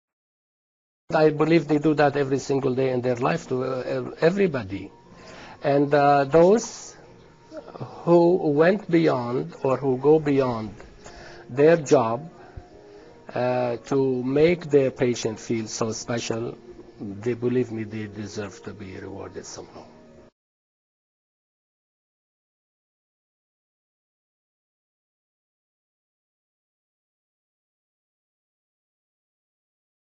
Health and Home-Award
Do you know a nurse who has really gone the extra mile?  Nominations are being invited for two new nursing awards that will be presented at the annual London city-wide nursing gala. They were initiated by spa owner Fayez Tamba.  When Tamba underwent thyroid surgery at Victoria Hospital last year, he was so impressed with the care and dedication of the nurses that he decided to create the nursing awards.